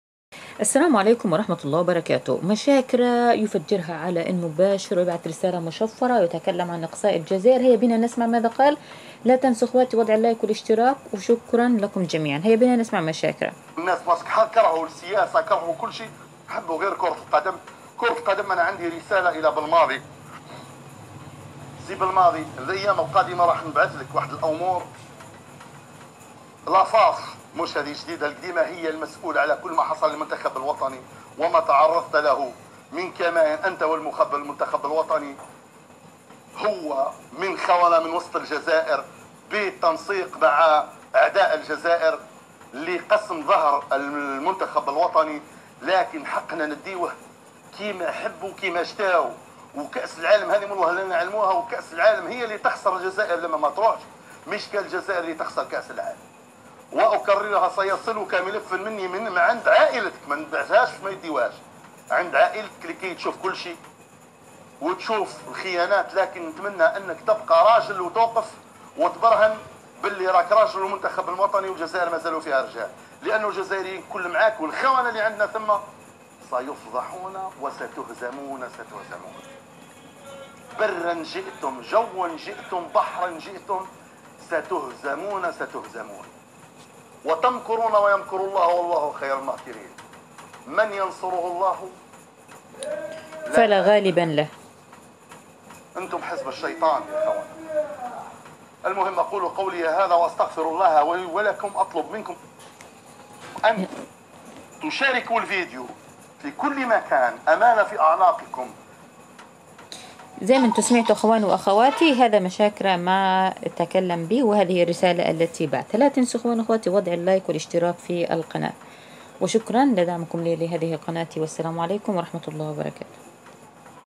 عاجل/مشاكره يفجر😱🔥ها ويعلنها برساله رسميه الى بلماضي
يوميات_شاديه_العموري مشاكره يخلطها وبعلنها برساله الى جمال بلماضي ويتكلم بامور مهمه